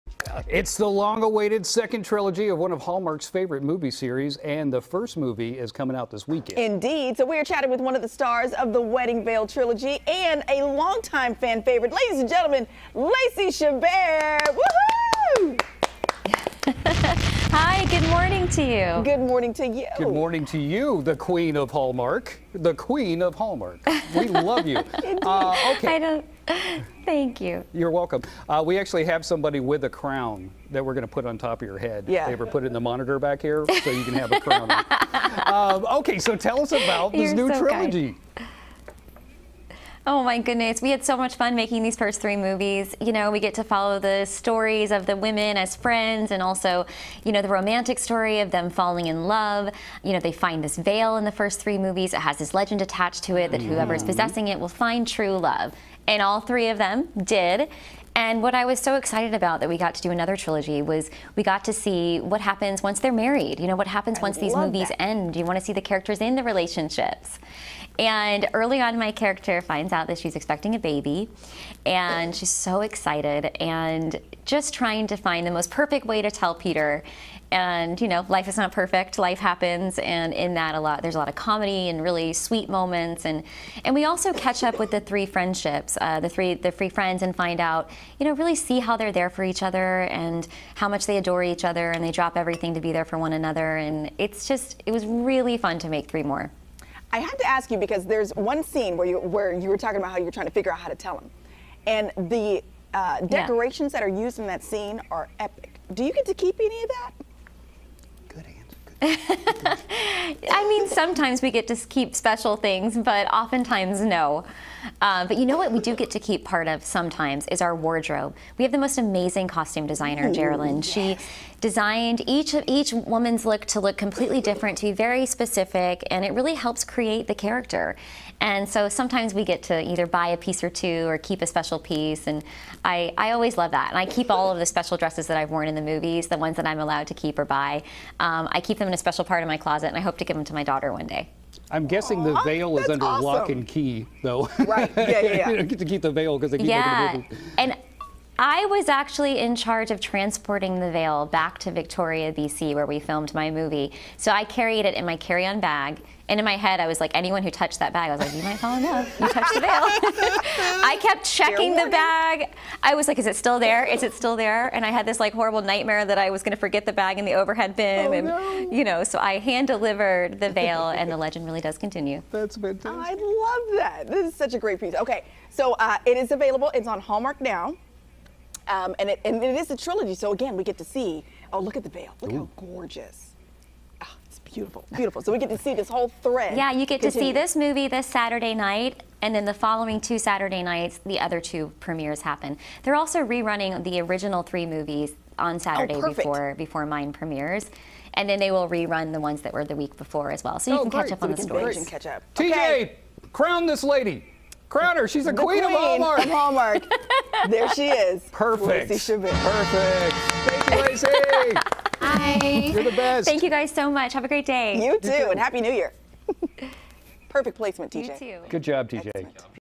"The Wedding Veil" star Lacey Chabert joins us!
There's a new "Wedding Veil" trilogy on the Hallmark Channel, and it brings back all of the stars from the original! Lacey Chabert is one of those stars, and she joins Cody and Courtney to tell us about the new movies! Also, we officially crown Lacey "The Queen of Hallmark!"